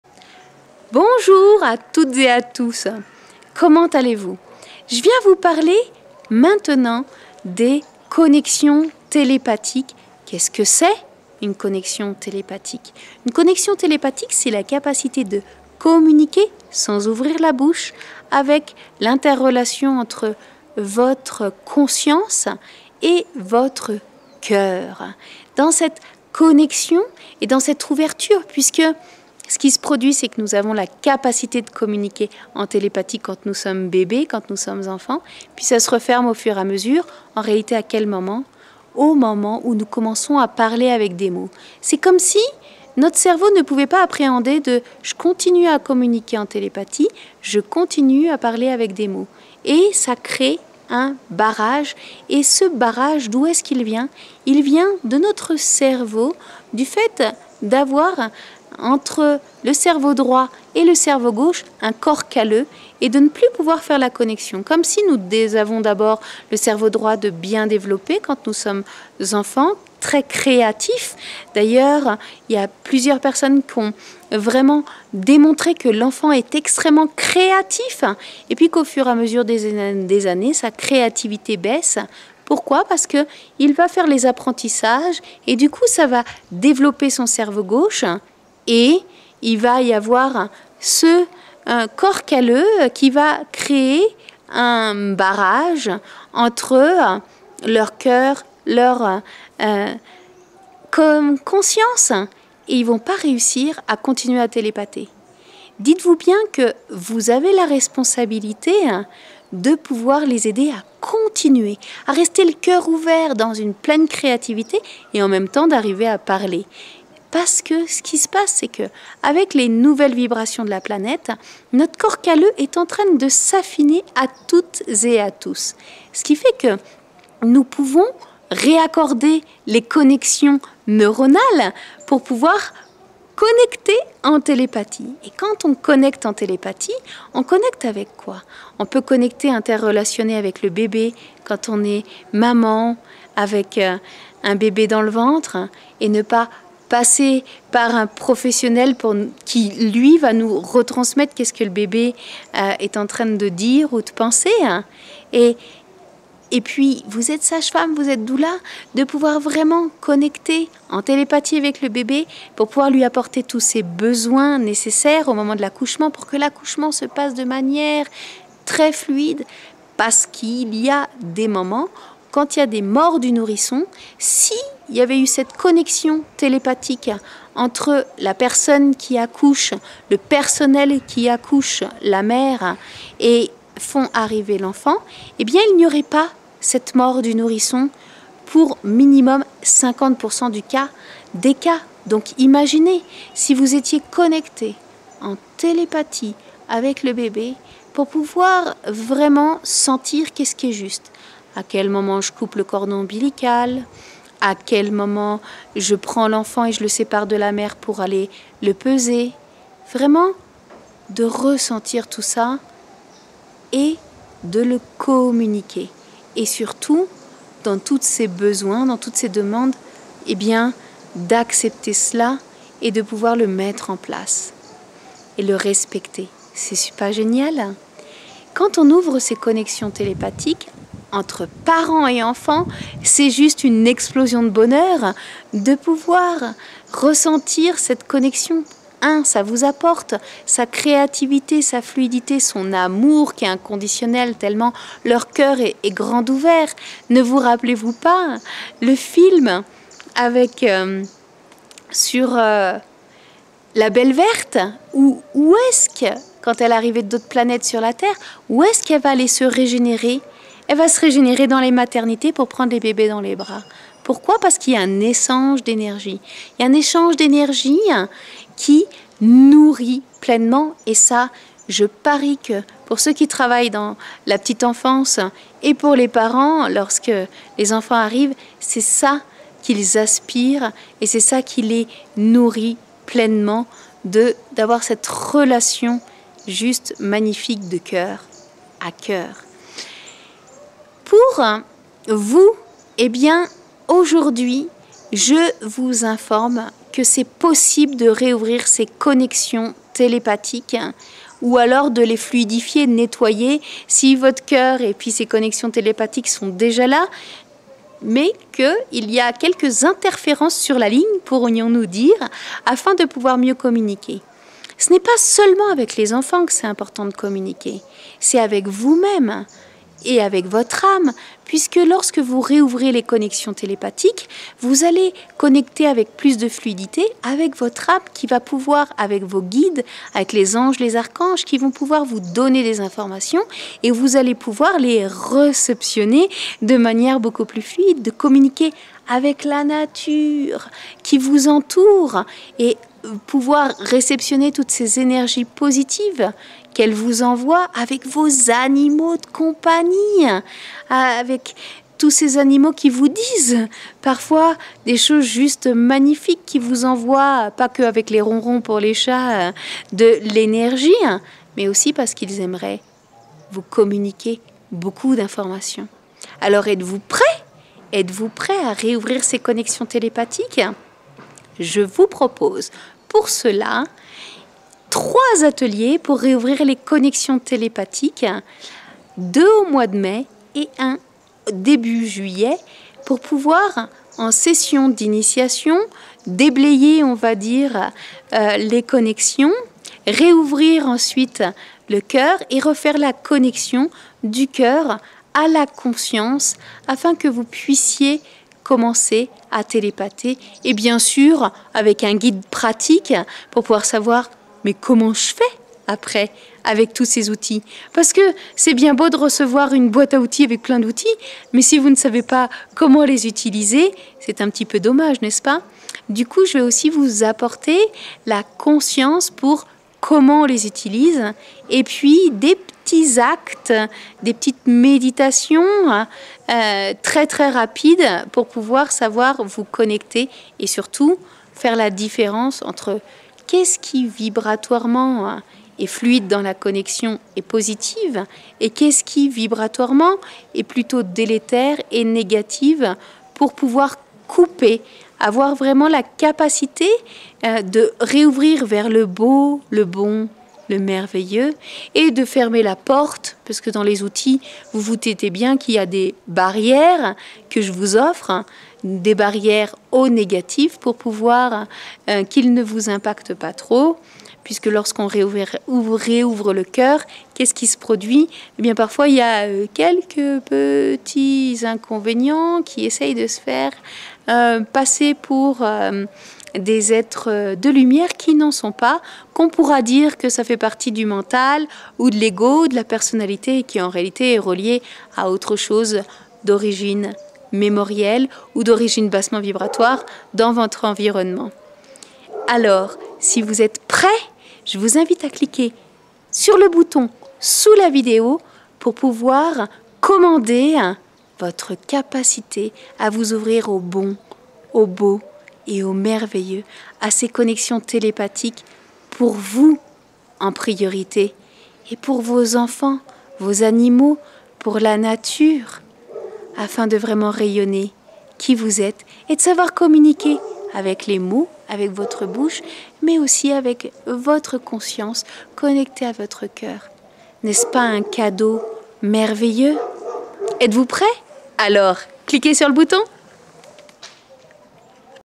Ateliers ouverture des connexions télépathiques
La télépathie est un moyen de communication que nous avons perdu et qu'il est important de se réapproprié pour mieux communiquer avec les enfants, la nature, les animaux.

Si vous aspirez a participer aux ateliers d'ouverture de  connexions télépathique a partir de 6 mai 2019 en 3 ateliers pour plus d'informations et inscription veuillez cliquer sur ce lien: